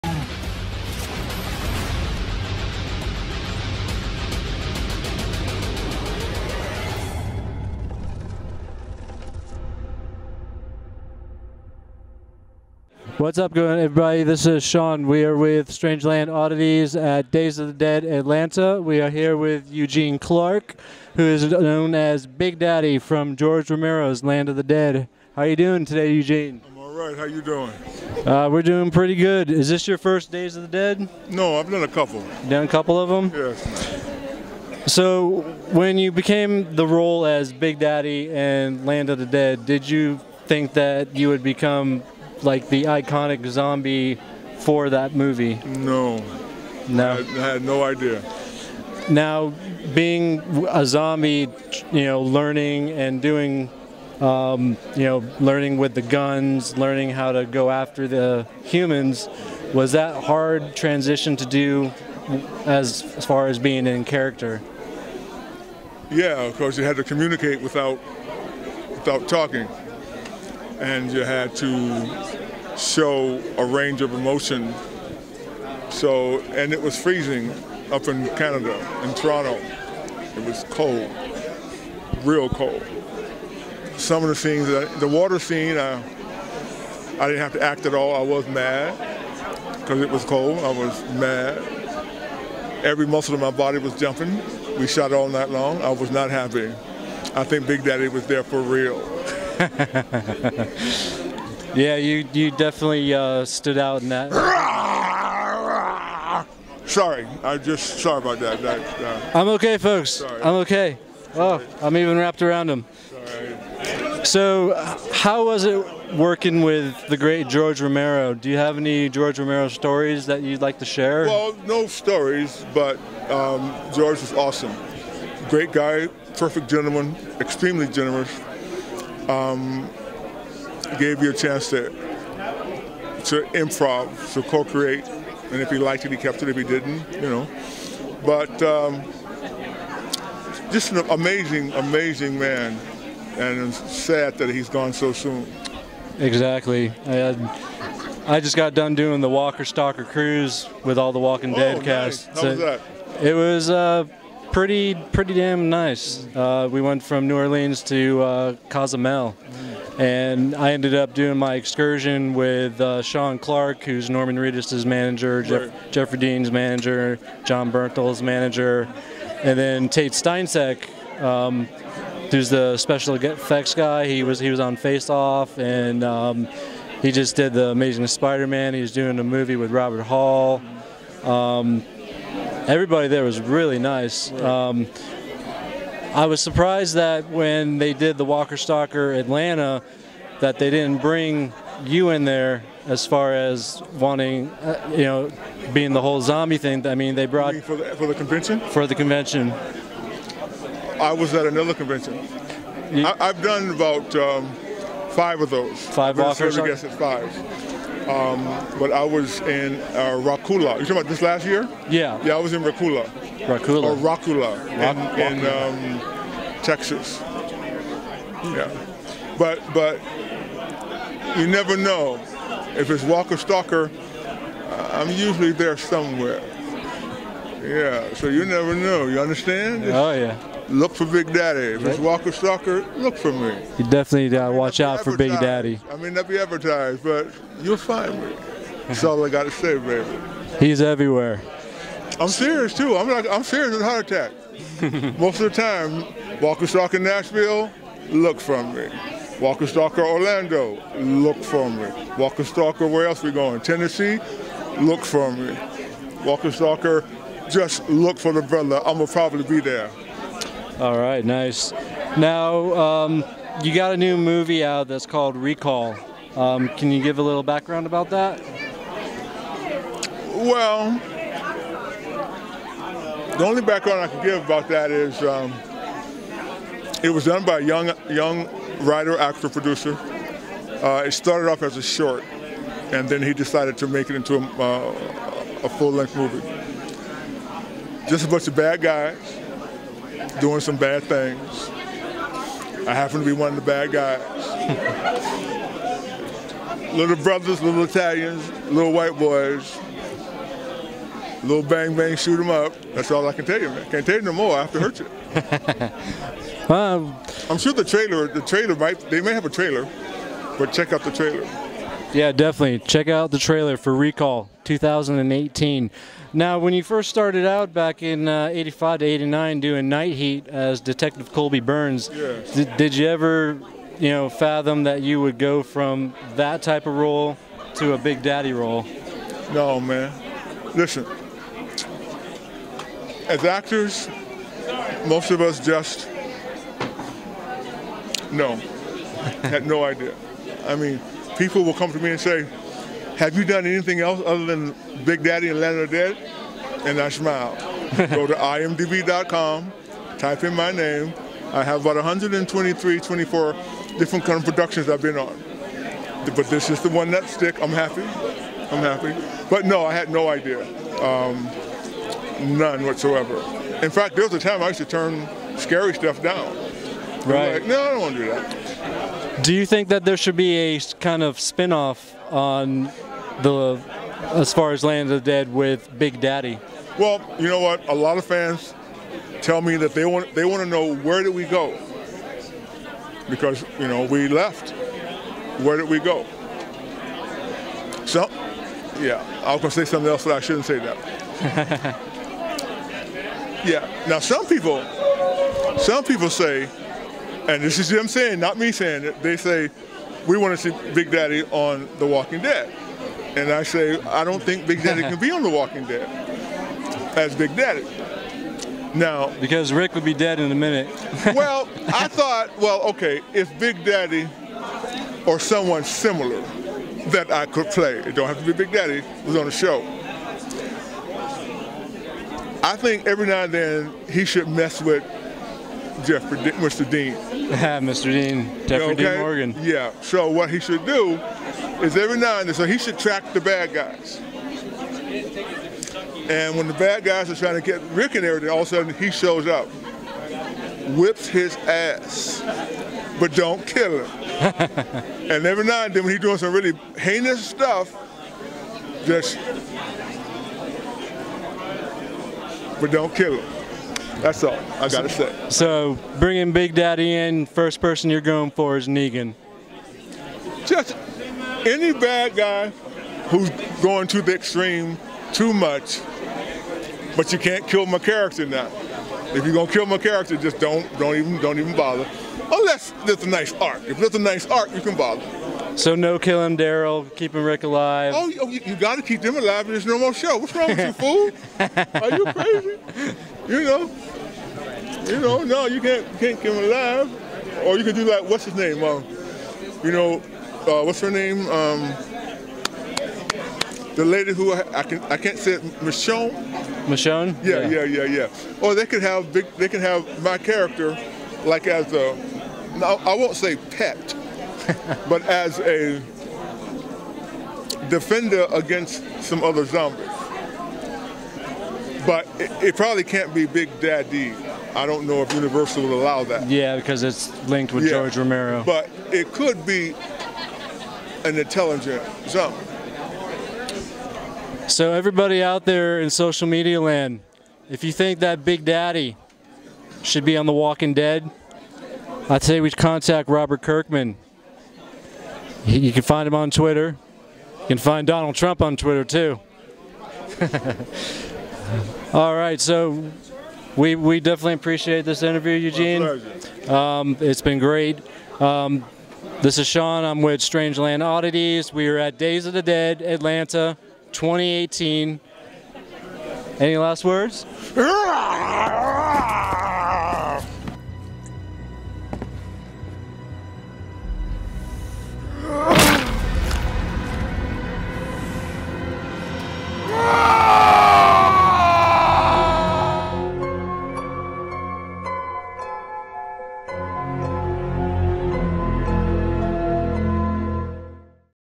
We got Attacked by Big Daddy aka Eugene Clark of George Romero's Land of the Dead
Eugene Clark Interview Feb 2nd 2018 at Days of the Dead Atlanta

Eugene was dressed up as BIG DADDY from George Romero's Land of the Dead. He described some great scenes, talked about his upcoming movie and what it be like if a character like BIG DADDY was put on The Walking Dead...The way the series goes anything can happen but during our interview I was ATTACKED twice by this big scary nice man. One hell of a guy

Interview and Produced by
Shaun Blayer

Edited by
Hunter Wayne

StrangeLand Oddities

“ All Things Strange and Odd “

StrangeLand Oddities

510 US Highway 98 N
Lakeland, Florida 33809